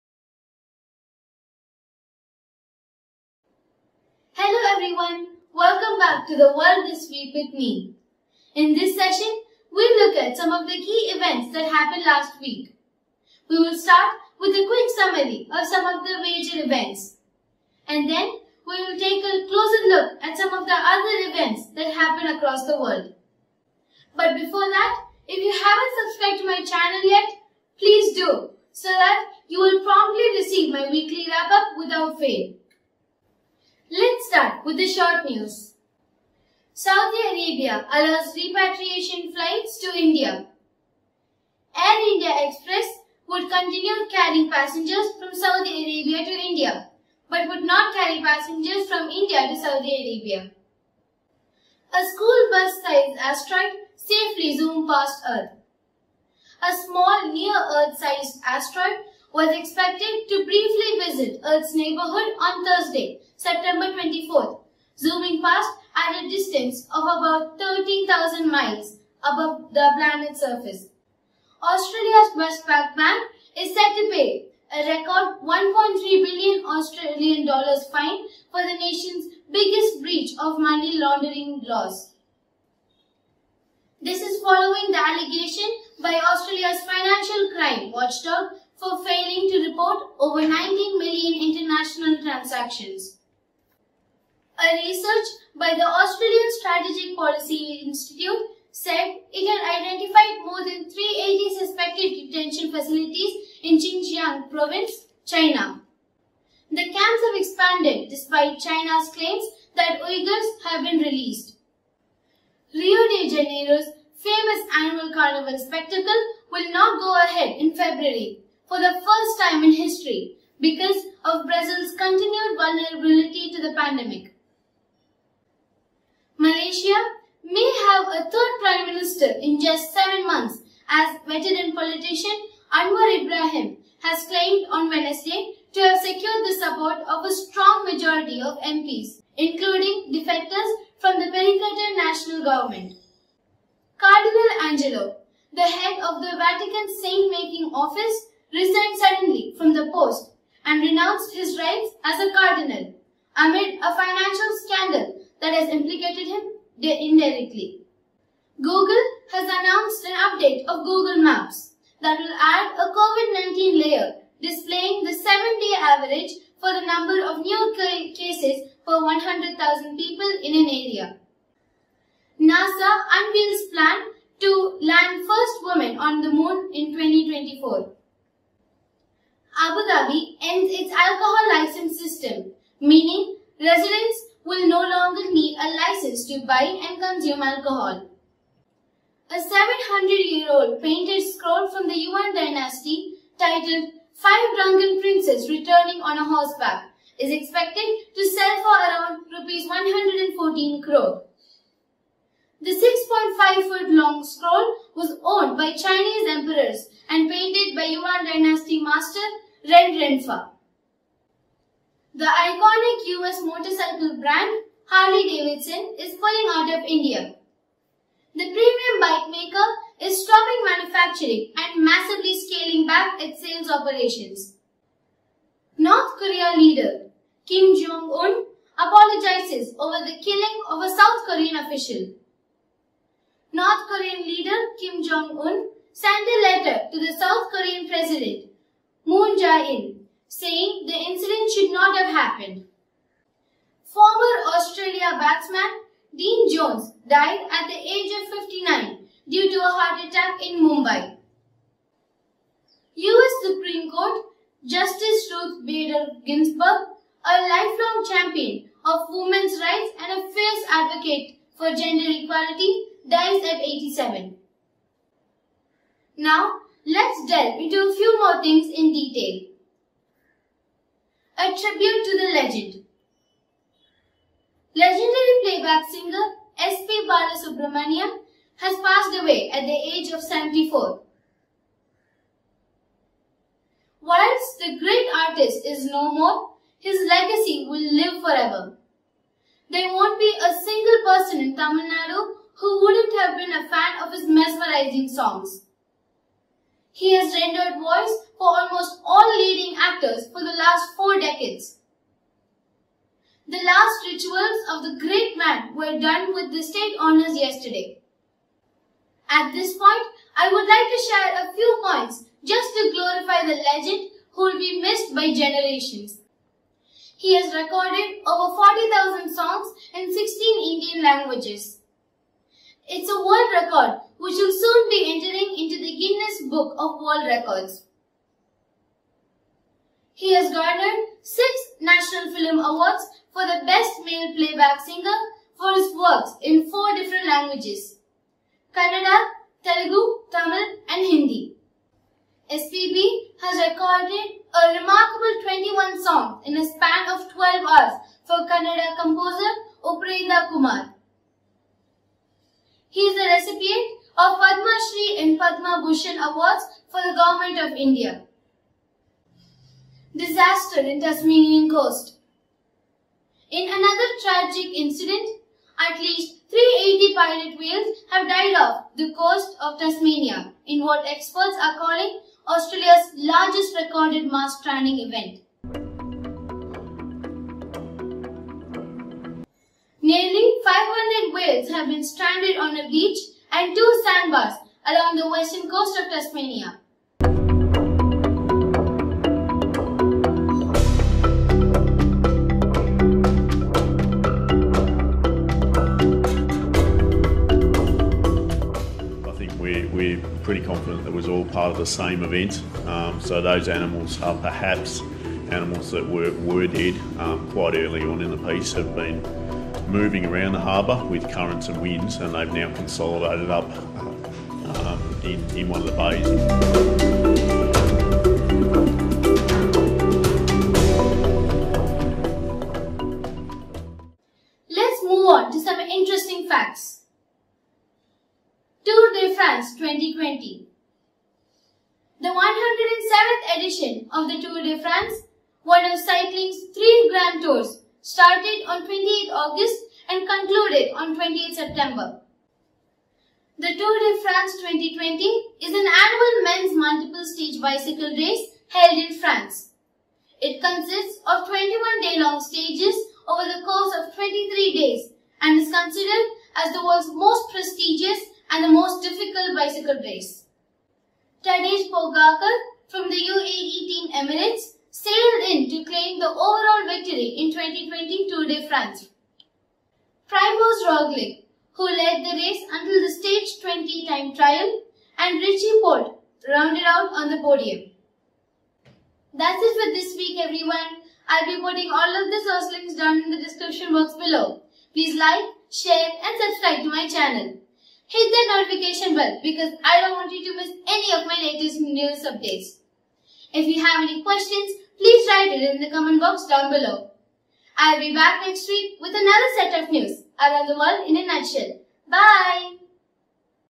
World This Week | 27-09-2020 | SPB's Legacy | 400 Whales Dead |Women to Moon | Korea Kim's Apologies
Saudi Arabia allows repatriation flights to India. Air India Express, would continue carrying passengers from Saudi Arabia to India, but would not carry passengers from India to Saudi Arabia

A School Bus-Size Asteroid Safely Zoomed Past Earth. A small near-Earth asteroid, was expected to briefly visit Earth's neighborhood on Thursday, Sept 24, zooming past at a distance of about 13,000 miles (22,000 kilometers) above our planet's surface

Australia's Westpac bank, is set to pay, a record 1.3bn Australian Dollars fine for the nation's biggest breach of money laundering laws. This is following the allegation, by Australia's financial crime watchdog for failing to report over, 19 million international transactions.

A research by The Australian Strategic Policy Institute said it had identified more than 380 suspected detention facilities in Xinjiang province, China. The camps have expanded, despite China's claims that Uighurs were being released.

Rio de Janeiro's famous annual Carnival spectacle will not go ahead in February, for the first time in history, because of Brazil’s continued vulnerability to the pandemic.

Malaysia may have a third prime minister in just seven months as veteran politician, Anwar Ibrahim, has claimed on Wednesday to have secured the support of a strong majority of MPs, including defectors from the Perikatan Nasional government.

Cardinal Angelo, the head of the Vatican's saint-making office, resigned suddenly from the post and renounced his rights as a cardinal, amid a financial scandal that has implicated him indirectly.

Google has announced an update of Google Maps, that will add a COVID-19 layer displaying the seven-day average for the number of new cases per 1,00,000 people in an area

9.NASA Unveils Plan to Land First Woman on the Moon in 2024 ttps://

10.  Abu Dhabi ends its alcohol licence system, meaning residents will no longer need a licence to buy and consume alcohol
11. A 700-year-old painted scroll from the Yuan Dynasty titled 'Five Drunken Princes Returning On Horseback' is expected to sell for around ₹114 crore. The 6.5-foot-long scroll was owned by Chinese emperors and painted by Yuan Dynasty master, Ren Renfa.

12. The iconic US motorcycle brand, Harley-Davidson, is pulling out of India. The premium biker maker is stopping manufacturing and massively scaling back its sales operations.

13. North Korea leader, Kim Jong Un, apologises over the killing of a South Korean official. North Korean leader Kim Jong Un sent a letter to South Korean President, Moon Jae-in, saying the incident should not have happened: South Korea

14. Former Australia batsman Dean Jones died at the age of 59 due to a heart attack in Mumbai. Jones was in Mumbai as a commentator for the IPL 2020

15. US Supreme Court, Justice Ruth Bader Ginsburg, a lifelong champion of women's rights and a fierce advocate for gender equality, dies at 87